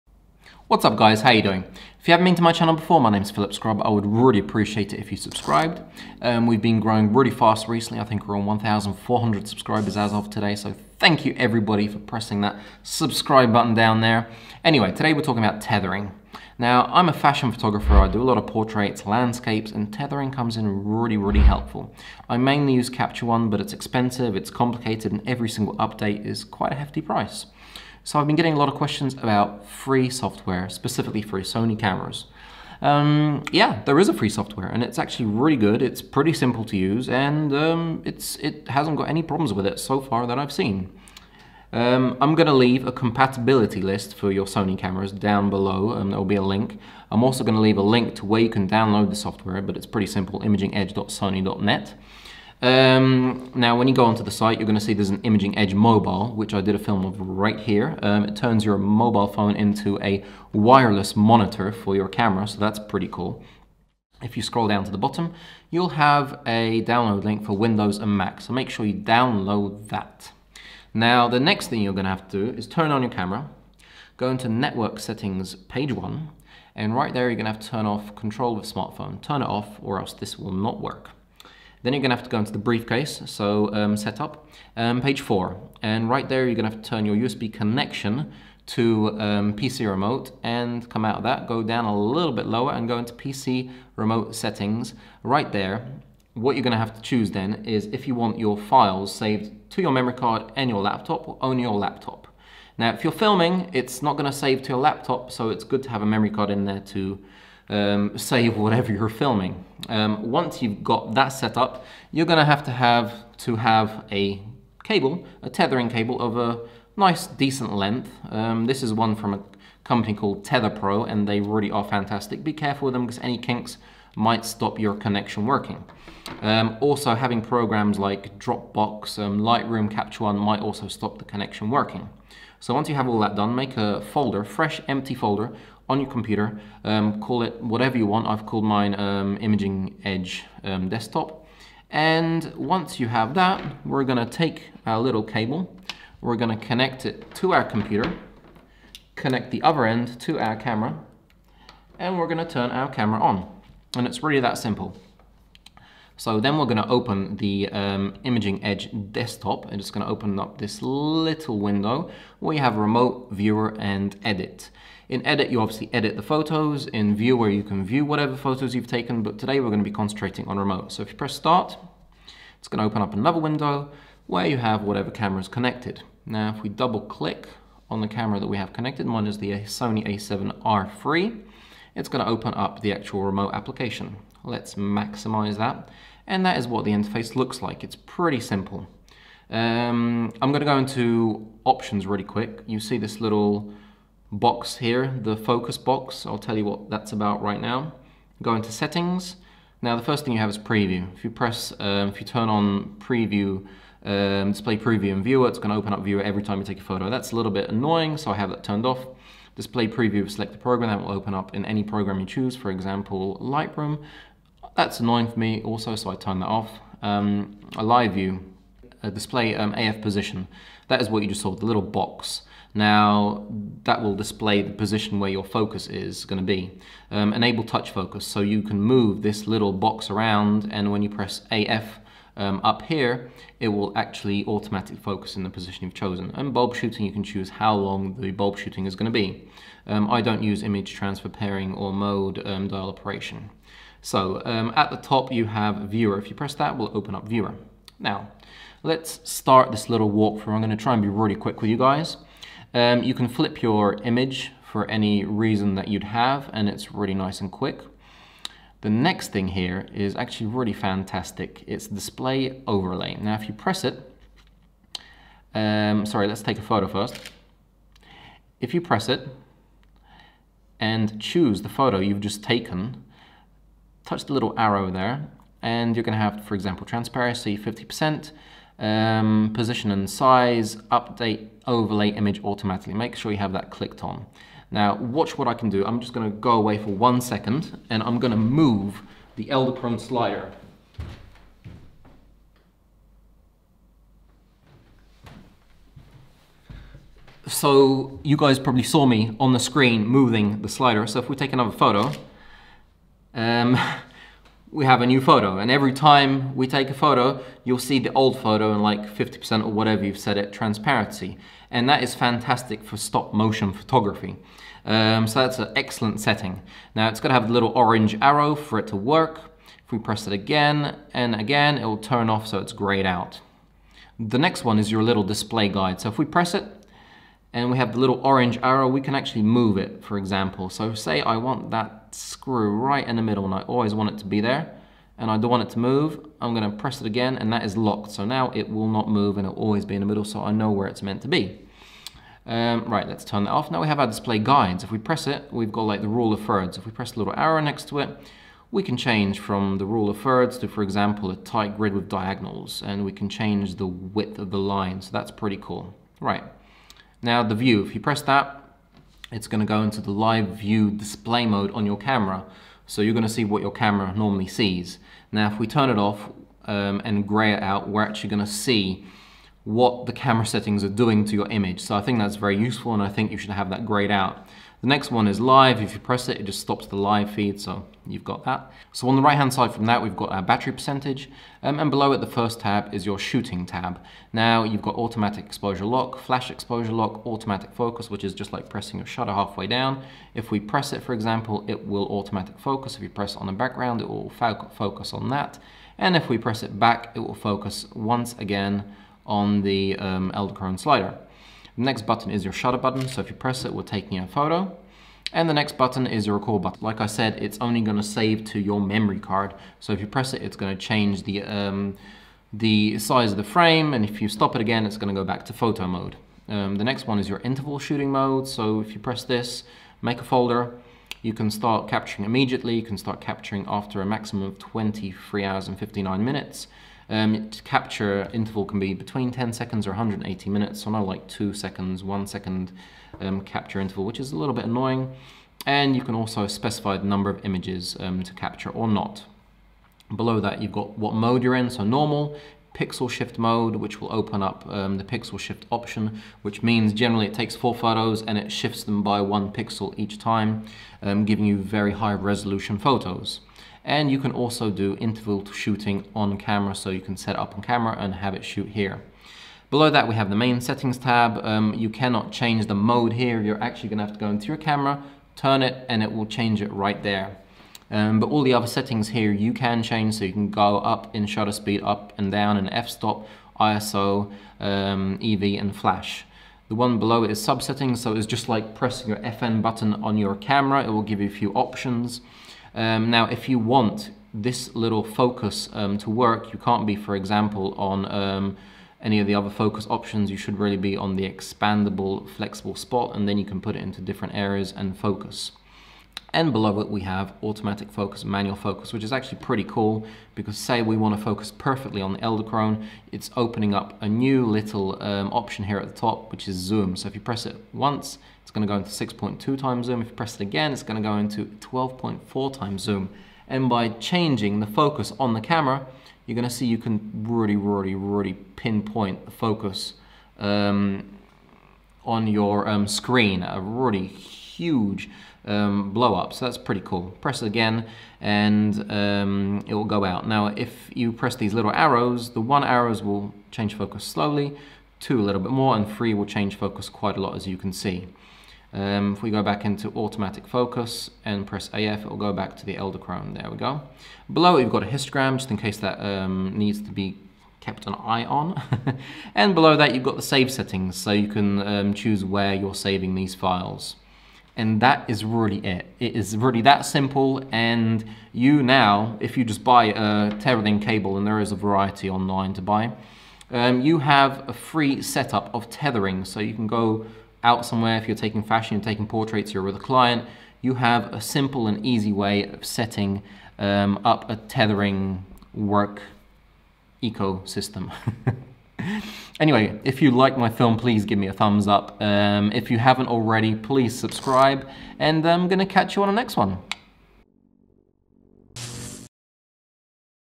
SONY IMAGING EDGE DESKTOP - How to TETHER | Sony A7iii A7Riii
SONY IMAGING EDGE DESKTOP -  HOW TO TETHER | Sony A7iii A7Riii. How to tether your Sony camera. It's simple, it's free and it's fast. Watch this film to find out how...

In this film I show you how to tether your Sony camera using Sony Imaging Edge Desktop software. This works for tethering photos and as a large view monitor for filmmaking.

CABLE:
TetherPro Amazon US
TetherPro Amazon EU

To tether your Sony camera I recommend using Tether Tools Tether Pro cables, they are perfect for tethering any camera.

If you have any questions at all about how to tether your camera, post them below in the comments and I'll make sure I get back to you.

⚡︎LUTS & PRESETS - Working on it...